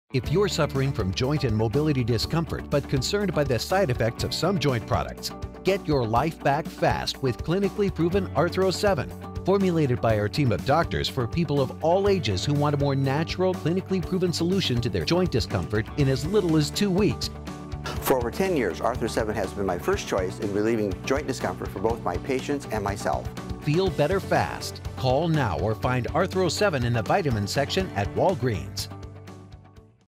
Arthro-7® Joint Health Supplement - TV Spot 30 Sec D
Advertisement for U.S. Doctors’ Clinical® - Arthro-7® Joint Health Supplement – Includes Doctor and Customer Testimonials and Reviews  *Now with 1-Year Money Back Guarantee